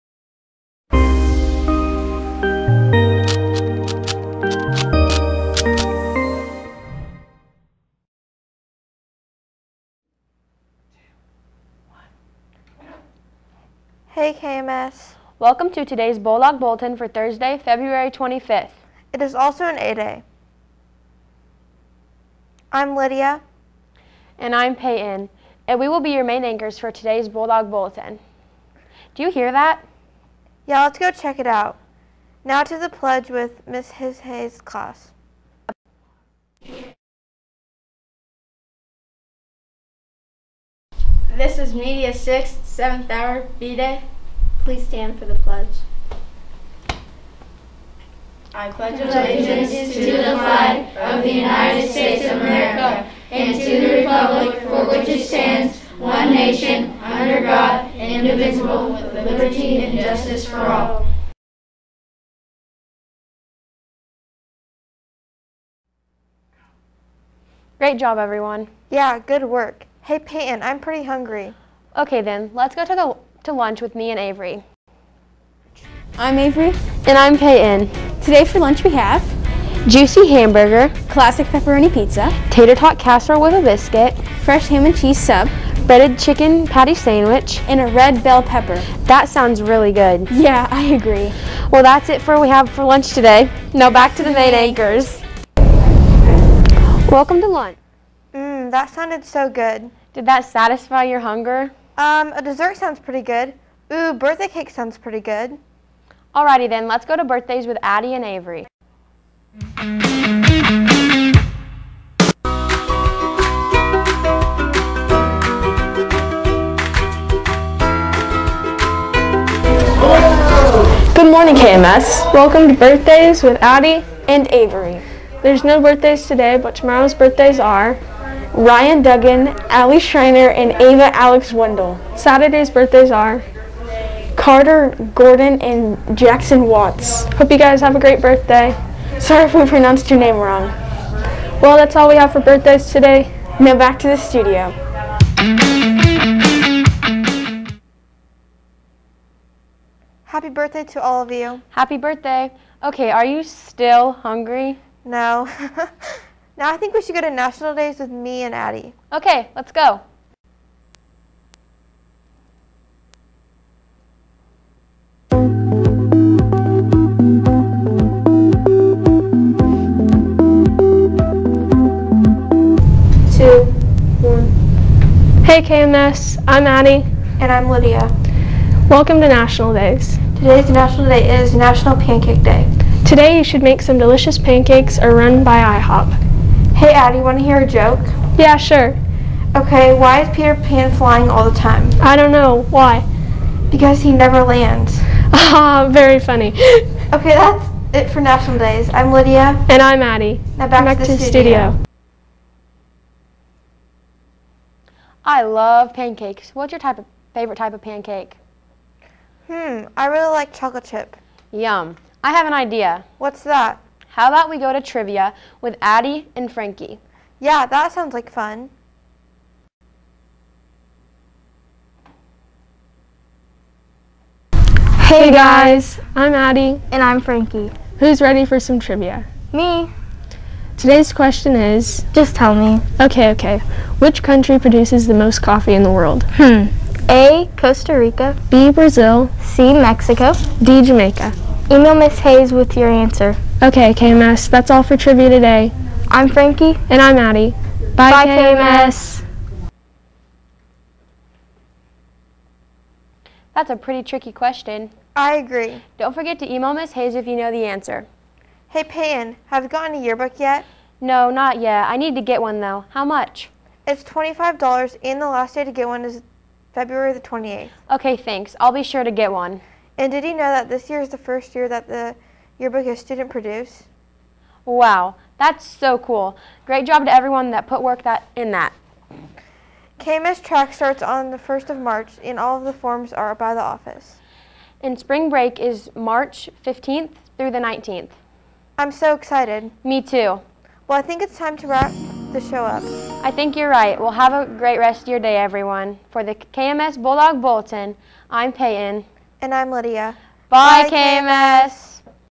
2-25-21
KMS Bulldog Bulletin Daily Announcements for 2-25-21
KMS Media 7 Students write, film, edit and publish the daily announcements- Bulldog Bulletin